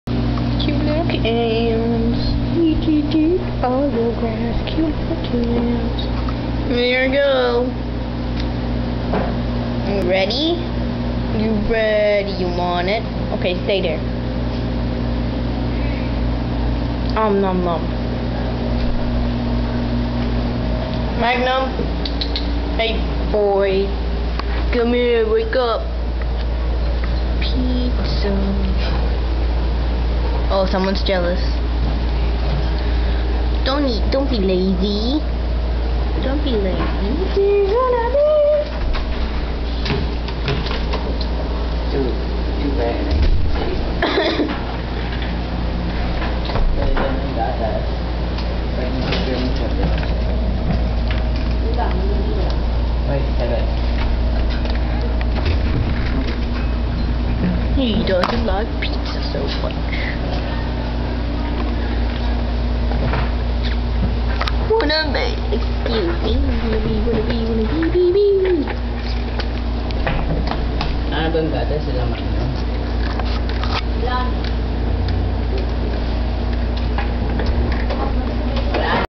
German Shepherds eating pizza crust
Magnum eats slow :) Wannabe is the gs at the beginning.